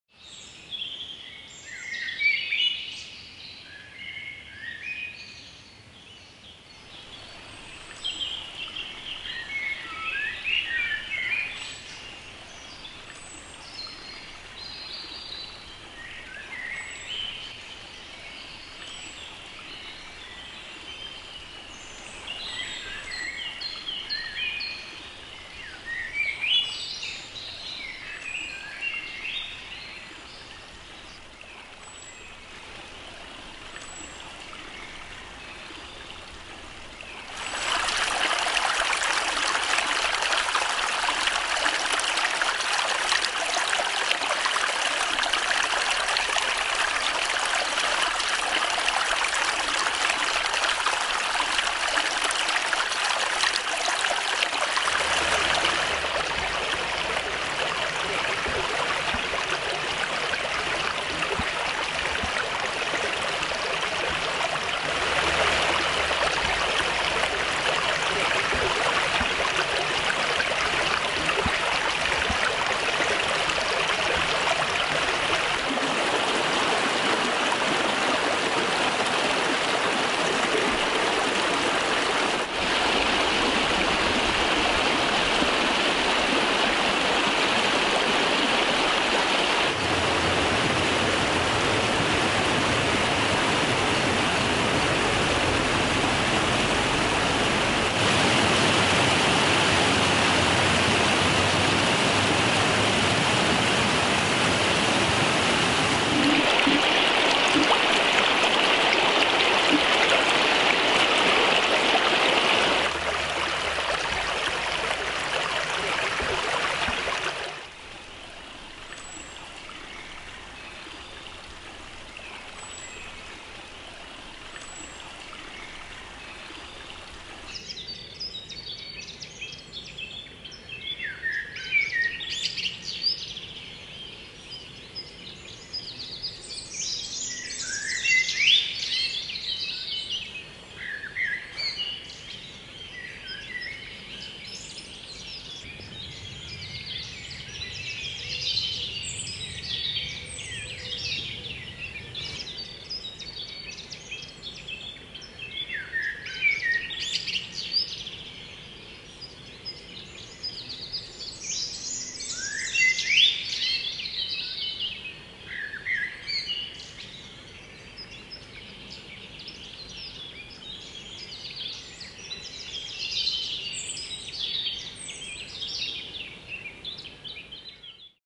Sunday morning, Wasserläufe im Odenwald
Germany:
Watercourses and landscapes, between Schönau and Hirschberg.

Wasserläufe und Landschaften, zwischen Schönau und Hirschberg an der Bergstraße.